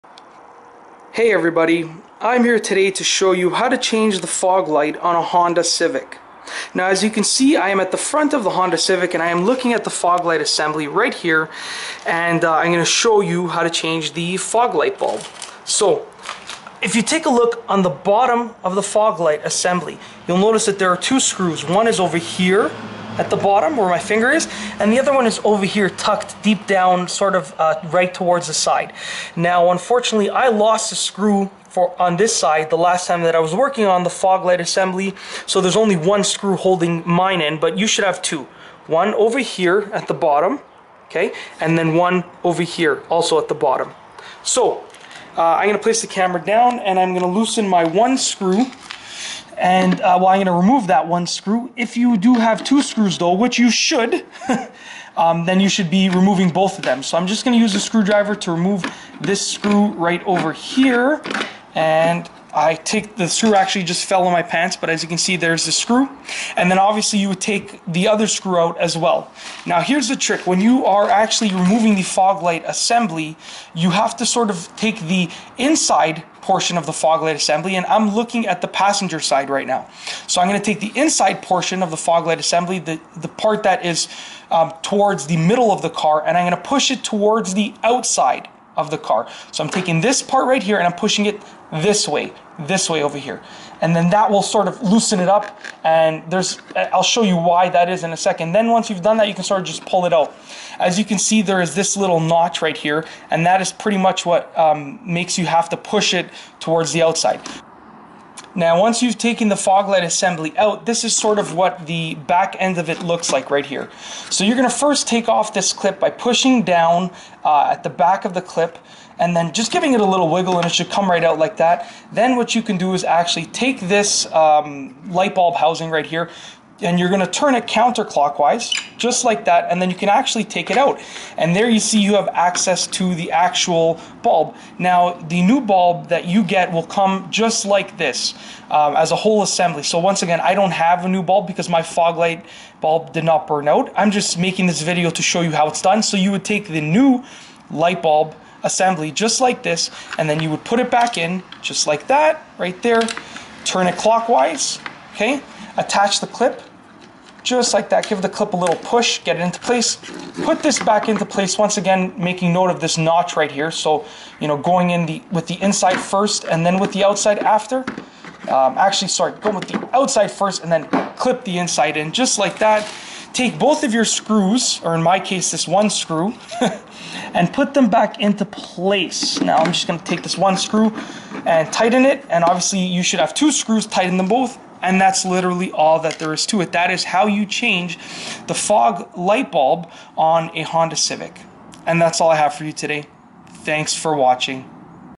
How To Change Foglight Bulb On 09-11 Honda Civic Coupe
In this video, I show you how to change the foglight bulb on a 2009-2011 Honda Civic. The foglights are very easy to access, and you can literally change a burnt out foglight in a matter of a few minutes. Its simply a matter of removing 2 screws, disconnecting a clip, and replacing the bulb.